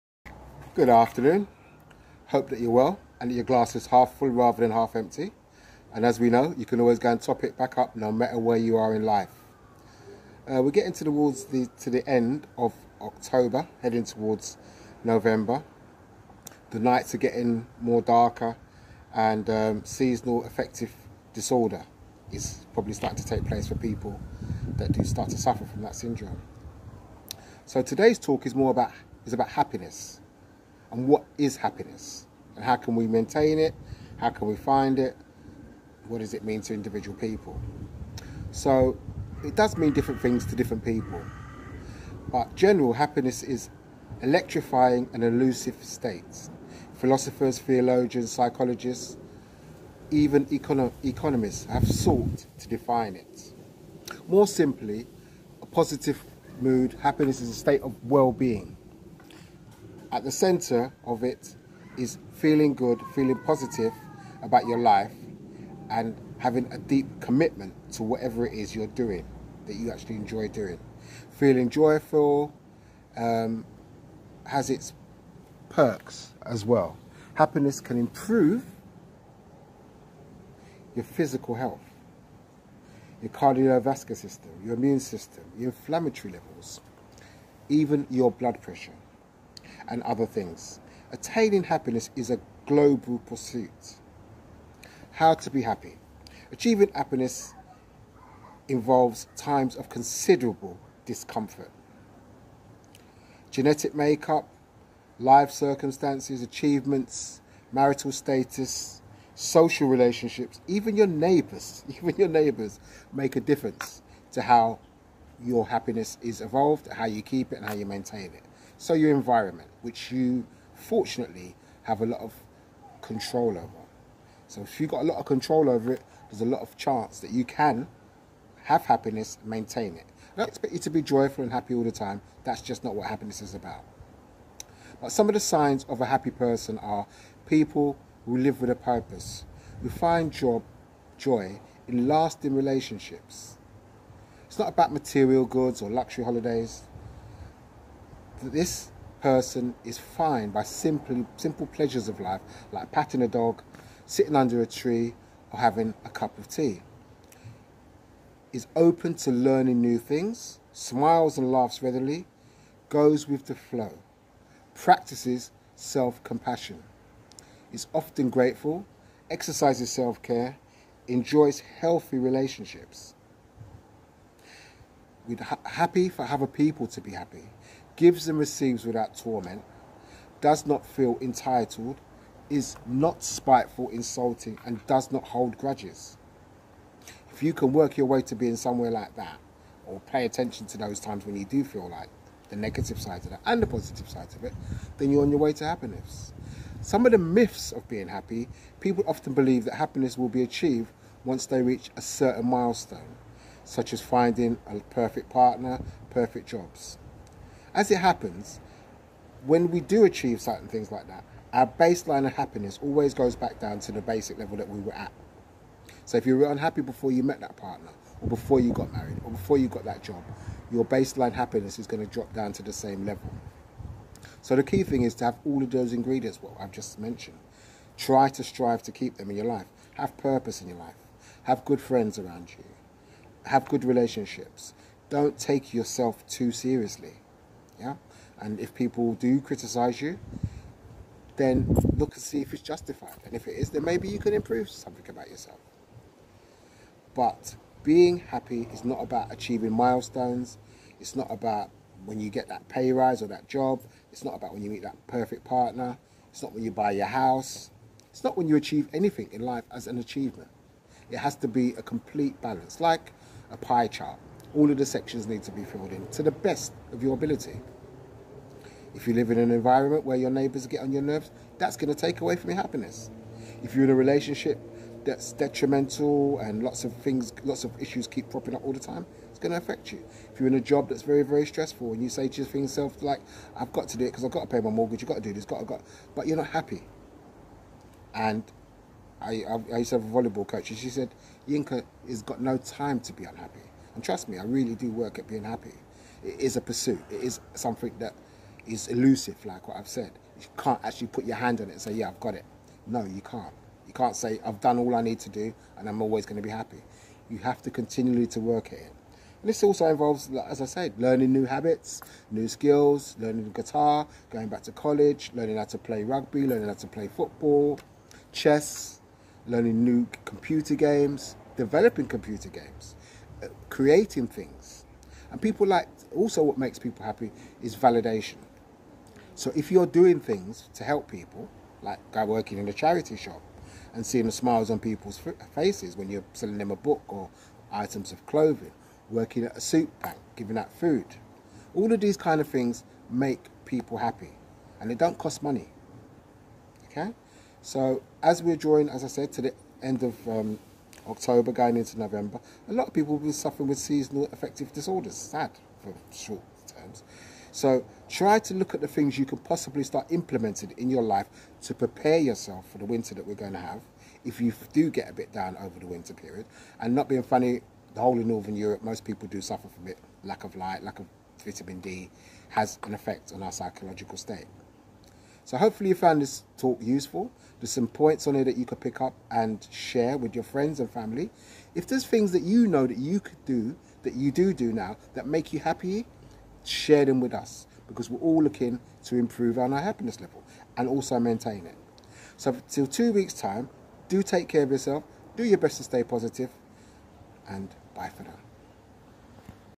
What is Happiness?
Happiness is an electrifying and elusive state. Philosophers, theologians, psychologists, and even economists have long sought to define it. And since the 1990s, a whole branch of psychology—positive psychology—has been dedicated to pinning it down. More than simply positive mood, happiness is a state of well-being that encompasses living a good life, one with a sense of meaning and deep contentment.